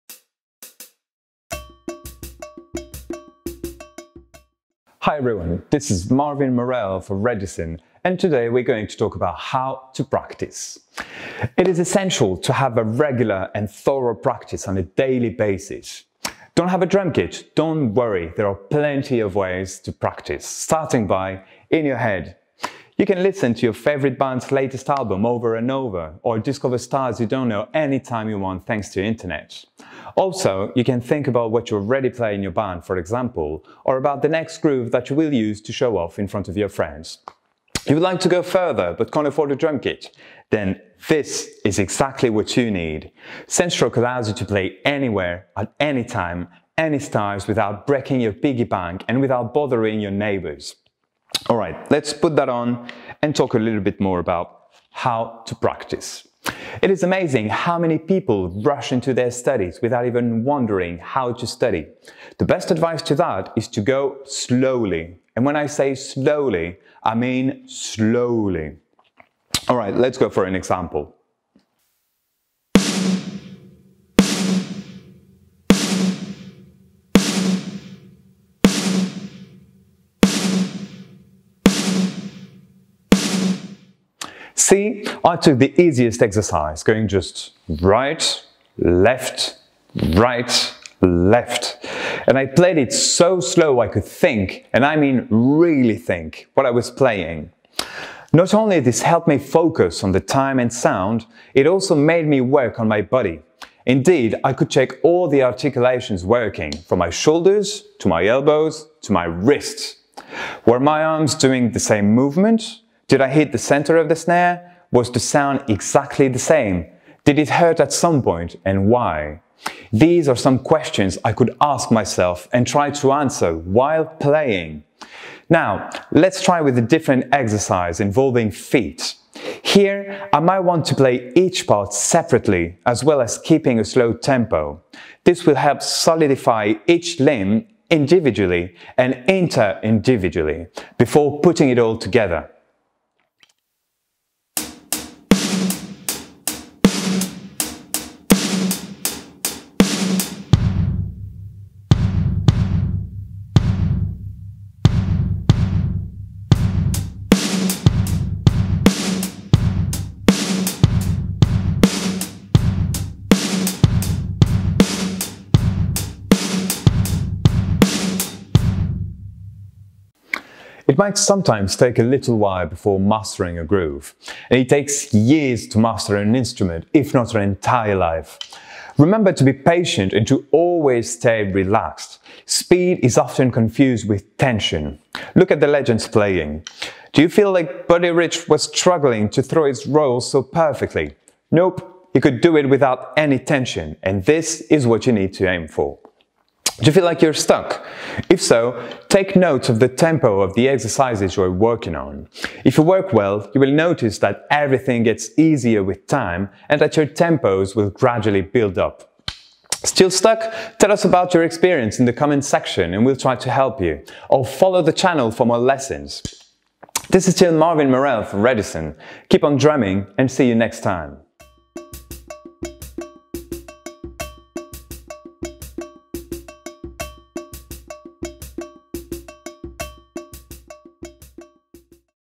Redison Drums Lessons #1 - How to practice with Marvin Morelle
Hello everyone !
We are very happy to present you our first Redison drum lesson with Marvin Morelle !
What do you think of it ? Do you want to see more ?
Amis francophones, pensez à activer les sous-titres en français !

Follow Redison !